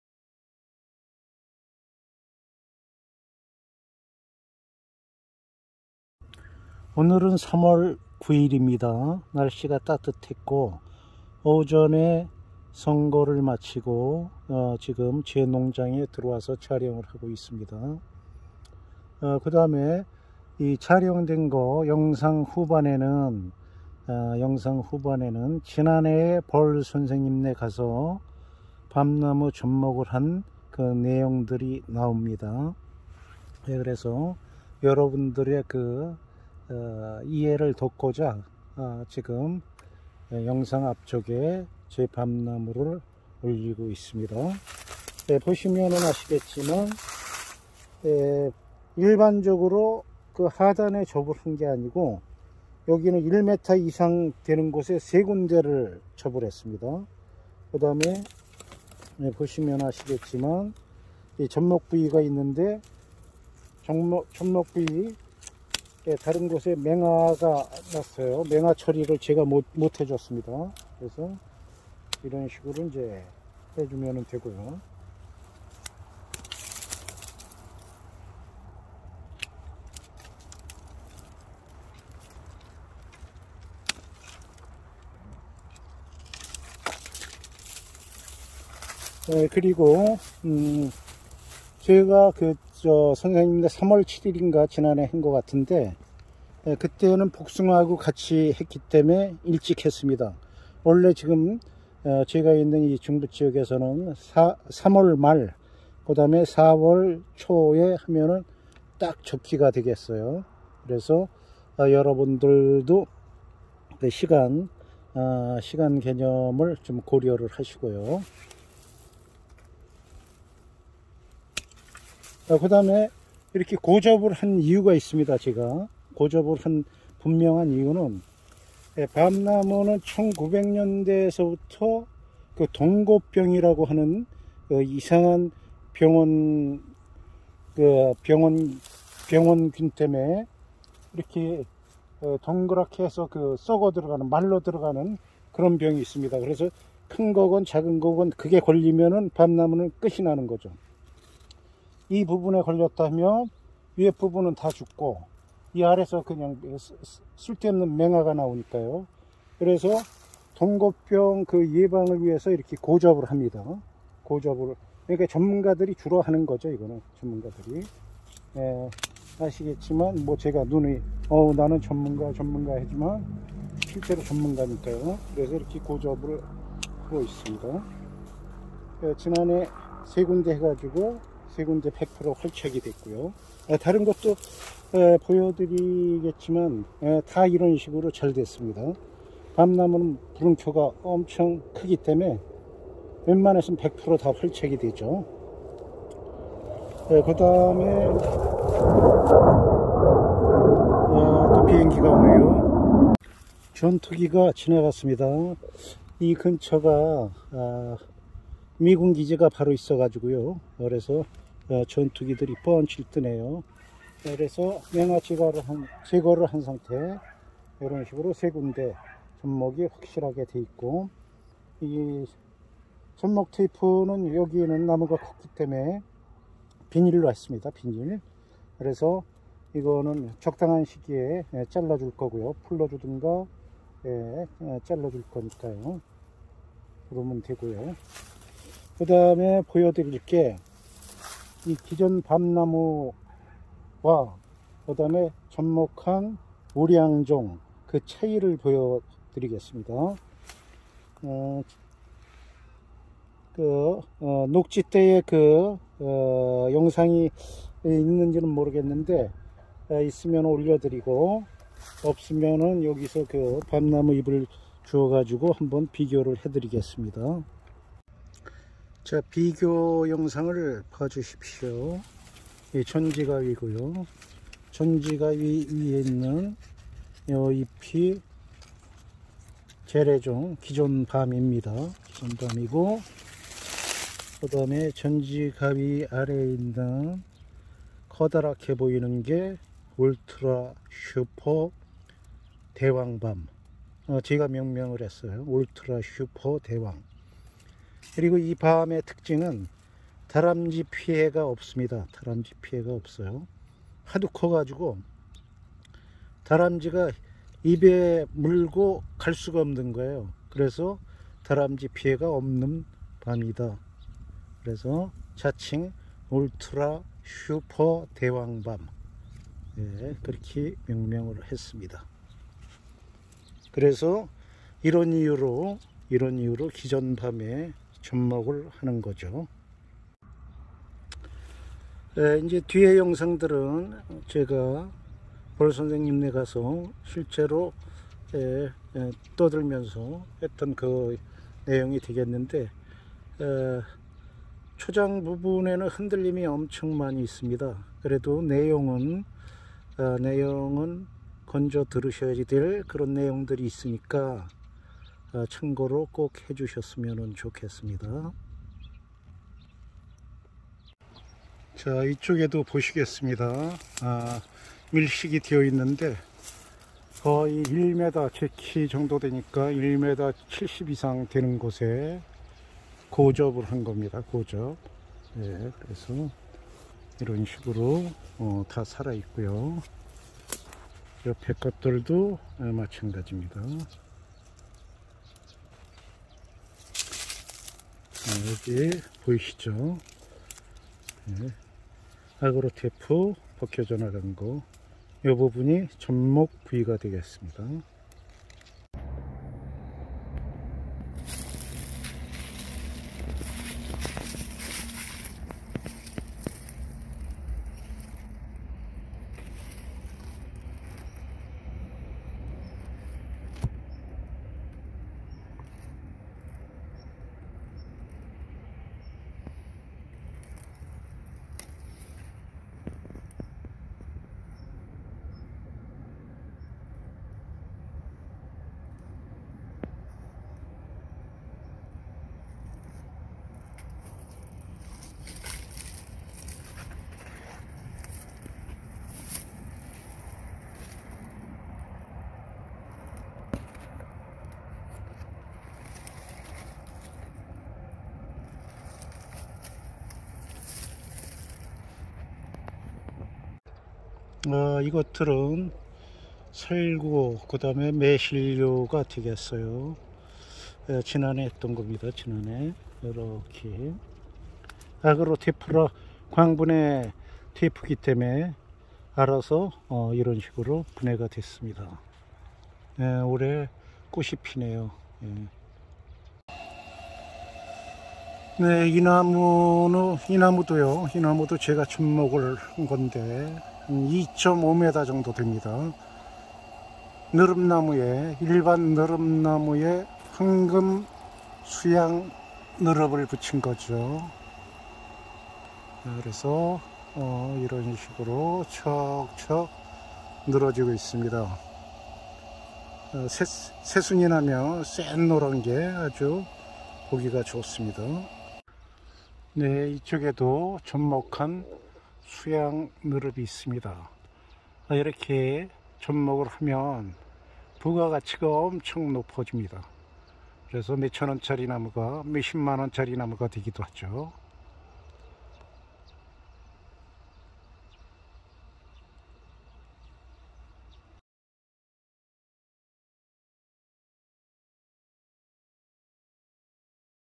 오늘의 접목...밤나무 접목후 1년
밤나무 접목후의 모습입니다..
영상에서 안내한 벌선생님댁의 원정 접목은 다음 영상에 있으니 참고하시구요.접목 현장모습이 담겨있으니 응용하셨음 합니다..